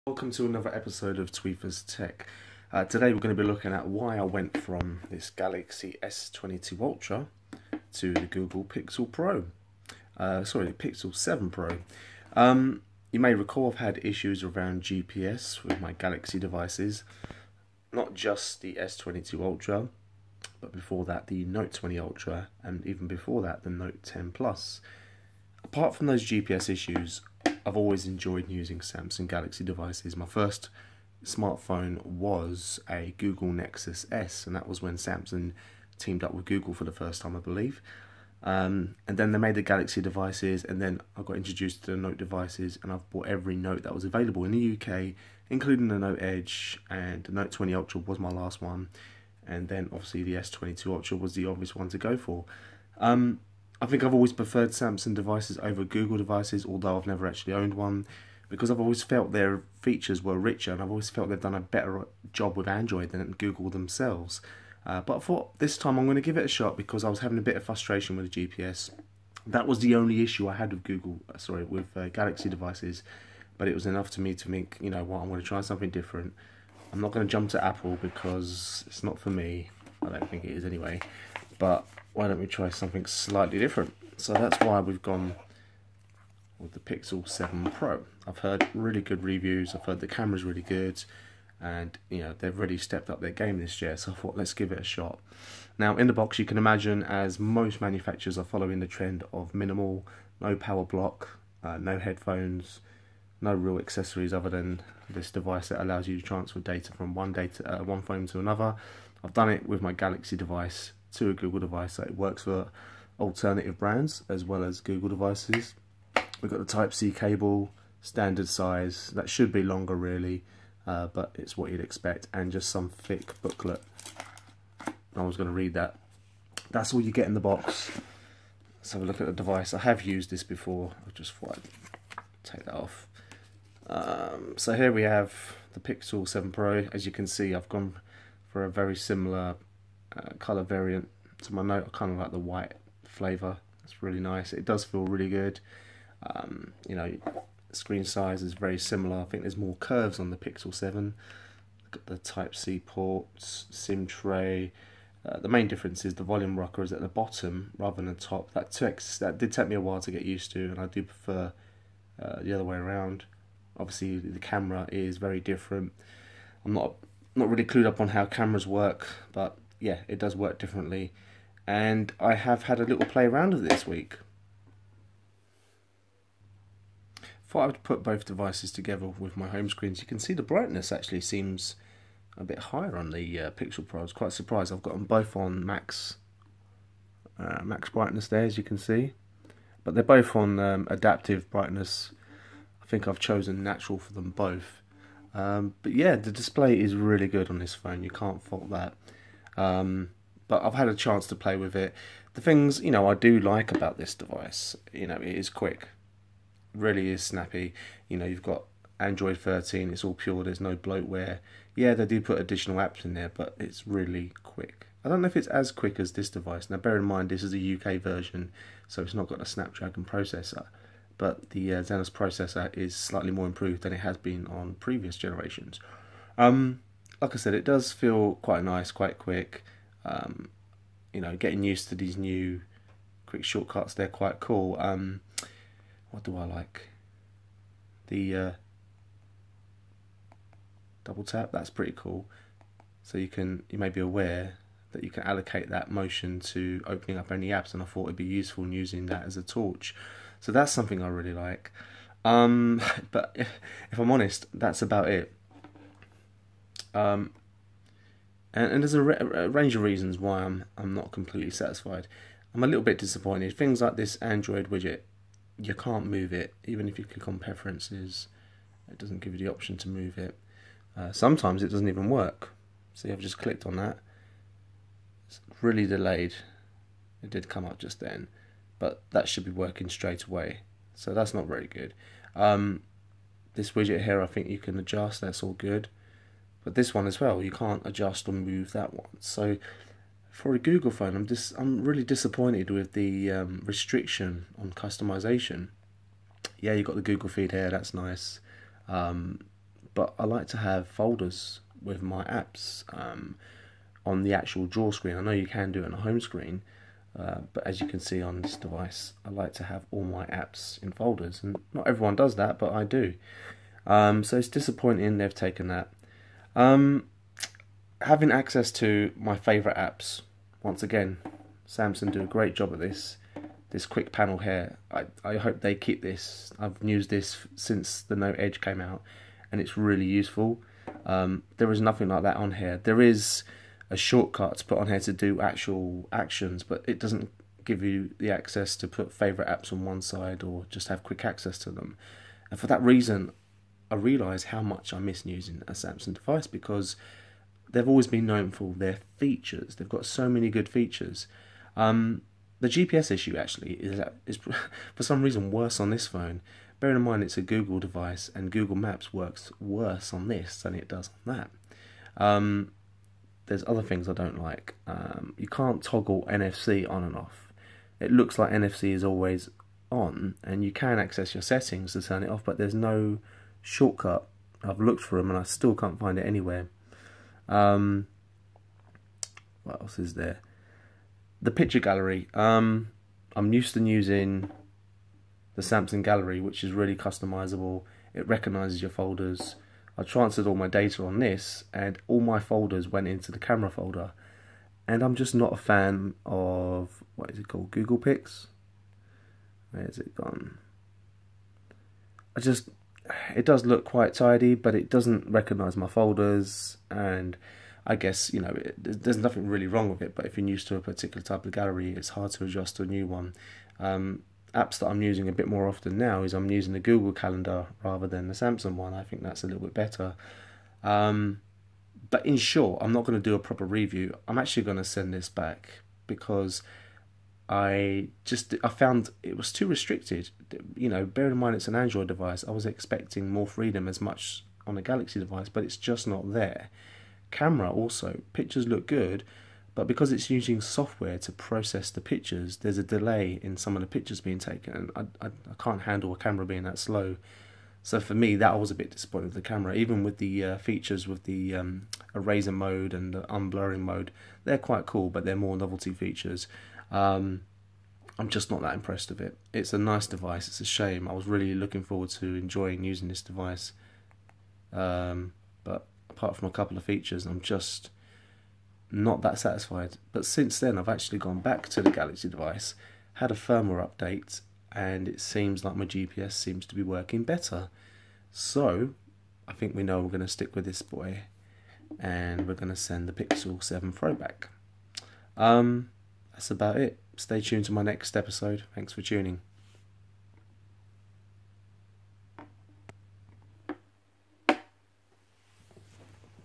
why I jumped from my galaxy S22 ultra to a Pixel 7 Pro, but jumped right back!
Was keen to try out the Pixel 7 pro but this video explains why I'm sending it back.
one of the things I've complained about, is the lack of customisations and removal of the google widget. People may suggest that 3rd party launchers can be downloaded to resolve that issue. i just can't be bothered to do that anymore as the one UI on galaxy devices is so good nowadays, I've not need to use alternative launchers. but it's still an option to consider.